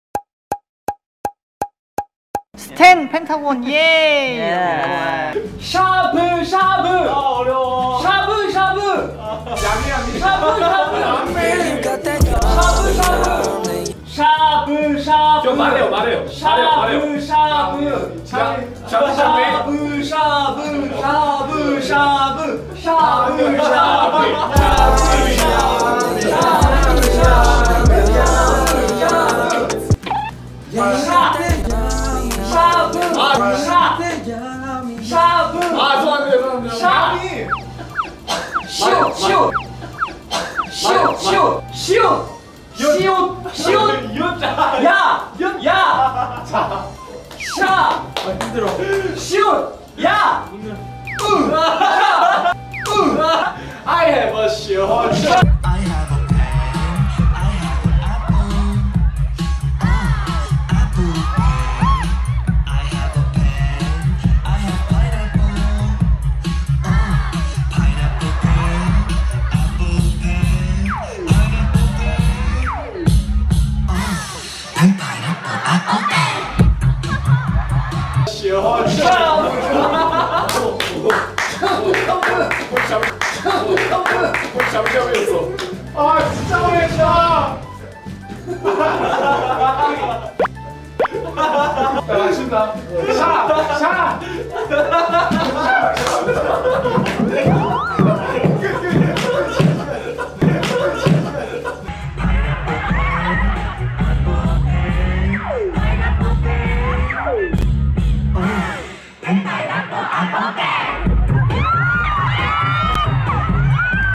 Hongseok & Shinwon's chaotic Shabu Shabu game
yummy yummy?